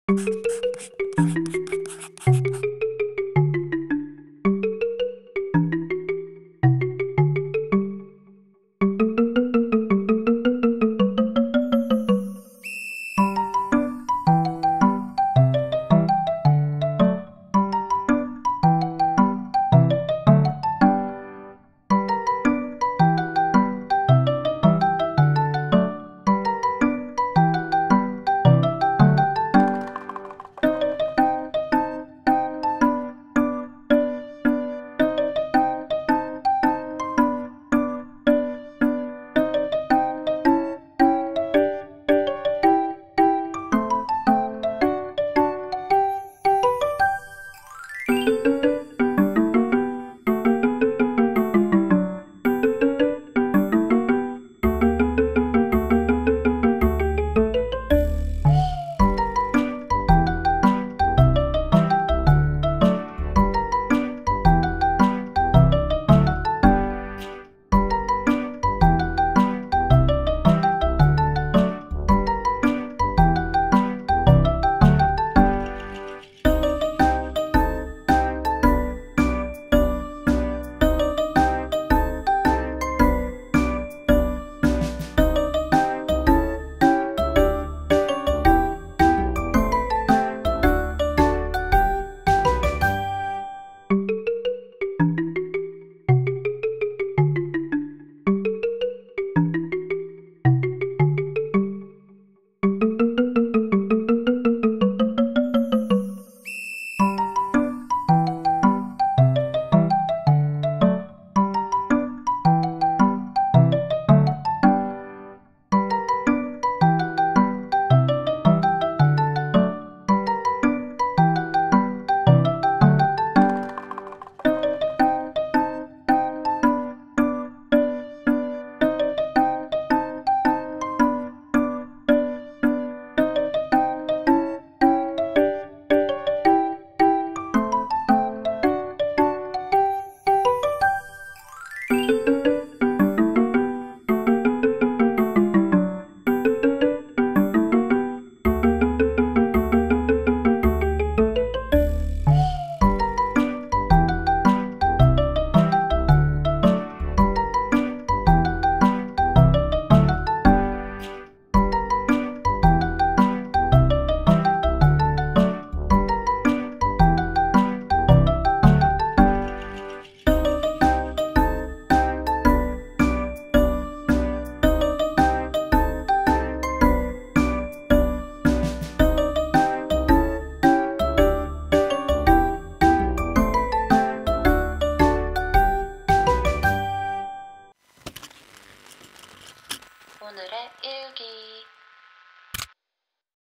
귀여운 음악 듣고 힘내세요 (Cheer up!)
가끔은 너무 지치는 하루가 있어요.
저는 그럴때 아주 단 디저트들을 먹어요.
그리고 음악을 틀어놓고 소파에 벌~러~덩 누워있는데요.
다들 스트레스를 어떻게 푸나요~?🐧